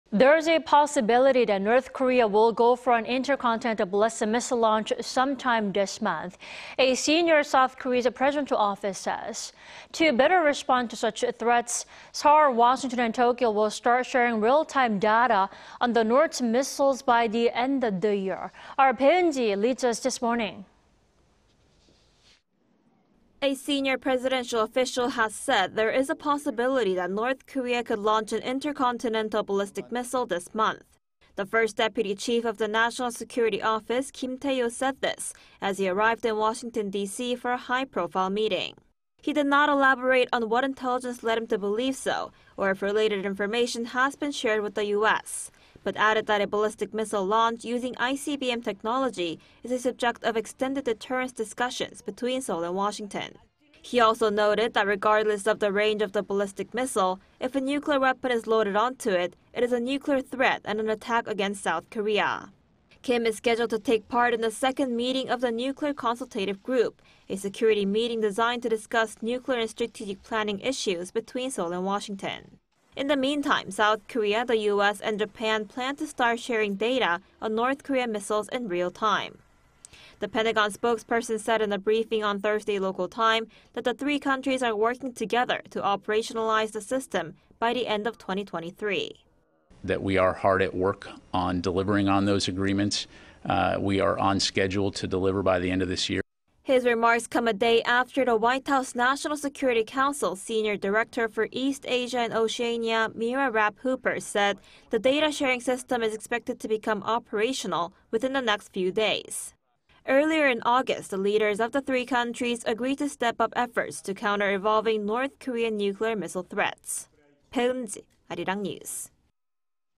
Senior S. Korean official says N. Korea could launch ICBM this month
안보실 1차장, "북한, 이달 중 ICBM 발사 가능성"... 美국방부 "한미일 미사일 정보공유 연내 가동 노력"

There is a possibility that North Korea will go for an intercontinental ballistic missile launch sometime this month, a senior South Korea's presidential office says.
To better respond to such threats, Seoul, Washington and Tokyo will start sharing real-time data on the North's missiles by the end of the year.
Bae Eun-ji leads us this morning.

A senior presidential official has said there is a possibility that North Korea could launch an intercontinental ballistic missile this month.
The first deputy chief of the National Security Office Kim Tae-hyo said this as he arrived in Washington D.C. for a high-profile meeting.
He did not elaborate on what intelligence led him to believe so, or if related information has been shared with the U.S. --but added that a ballistic missile launch using ICBM technology is a subject of "extended deterrence" discussions between Seoul and Washington.
He also noted that regardless of the range of the ballistic missile, if a nuclear weapon is loaded onto it, it is a nuclear threat and an attack against South Korea.
Kim is scheduled to take part in the second meeting of the Nuclear Consultative Group --a security meeting designed to discuss nuclear and strategic planning issues between Seoul and Washington.

In the meantime, South Korea, the U.S. and Japan plan to start sharing data on North Korean missiles in real-time.
The Pentagon spokesperson said in a briefing on Thursday local time that the three countries are working together to operationalize the system by the end of 2023.

"We are hard at work on delivering on those agreements. We are on schedule to deliver by the end of this year "

His remarks come a day after the White House National Security Council senior director for East Asia and Oceania, Mira Rapp-Hooper said the data-sharing system is expected to become operational "within the next few days."
Earlier in August, the leaders of the three countries agreed to step up efforts to counter evolving North Korean nuclear and missile threats.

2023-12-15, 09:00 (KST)